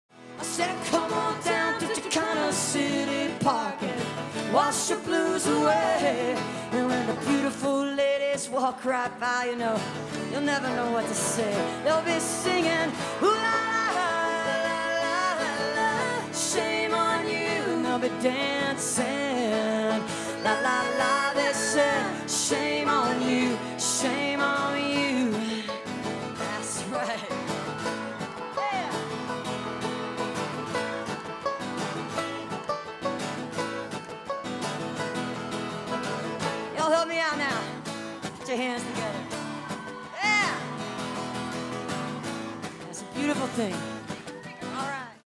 ACL 2007: Indigo Girls
Indigo Girls perform live on day 2 of ACL Fest 2007.